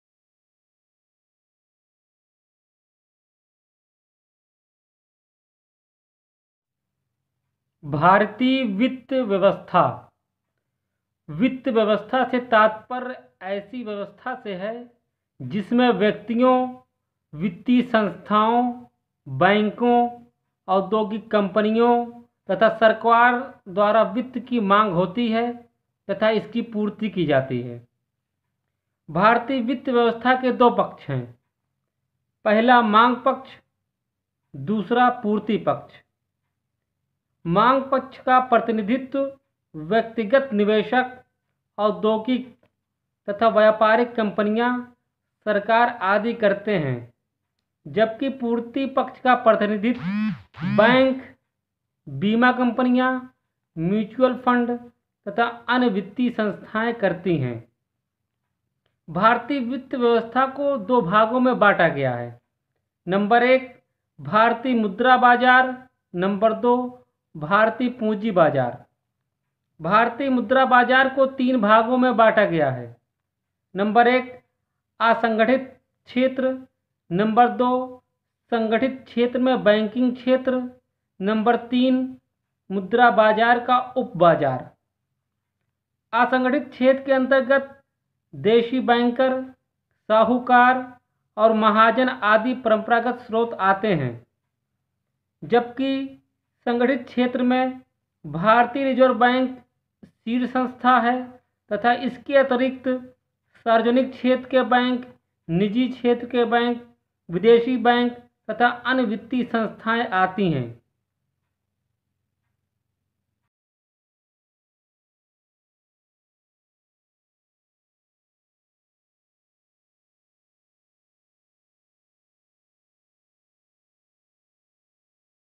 भारतीय वित्त व्यवस्था। एच.आई.ए कोचिंग सेंटर टांडा। इब्राहिम अंसारी
यह वीडियो टीईटी, सीटेट ,एसएससी ,रेलवे ,बैंकिंग की परीक्षा की तैयारी करने वाले विद्यार्थियों के लिए बनाई गई है इस वीडियो में भारतीय वित्त व्यवस्था के बारे में बताया गया है आप इब्राहिम अंसारी को फेसबुक इंस्टाग्राम टेलीग्राम पर भी फॉलो कर सकते हैं